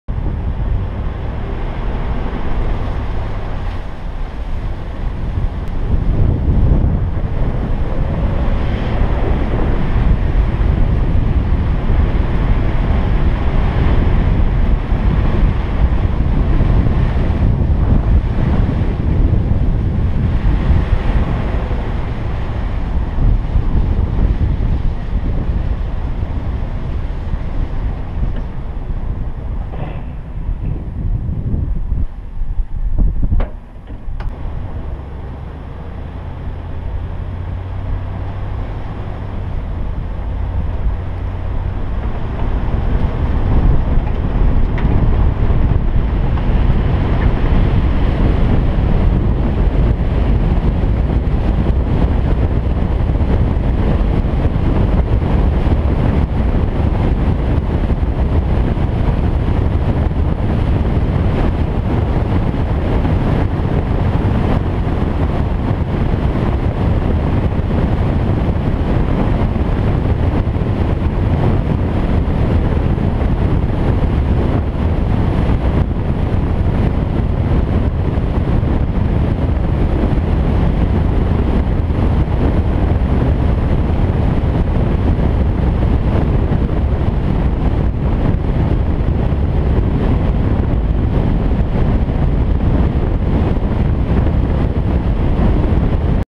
Bonneville Salt flats
Playing around driving on the (wet) Bonneville proving grounds.